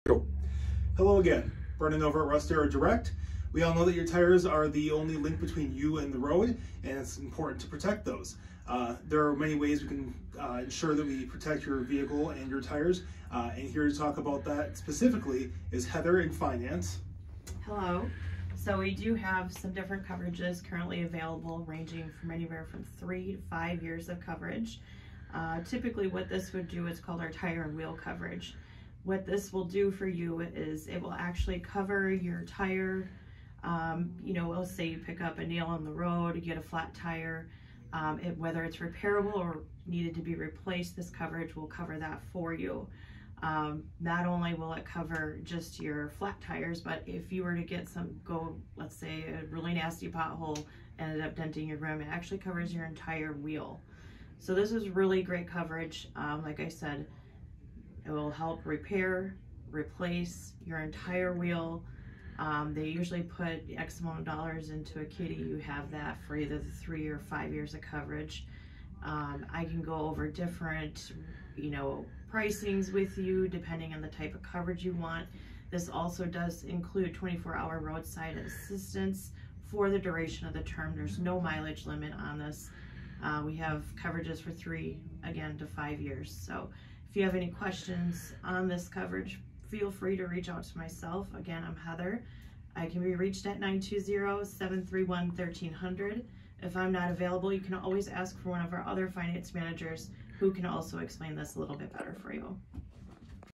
Tire and Wheel Protection at Russ Darrow Appleton Direct
Your tires are the only thing that stand between you and the road. Join Brennan and Heather as they discuss the ways we can help you protect your tires and wheels.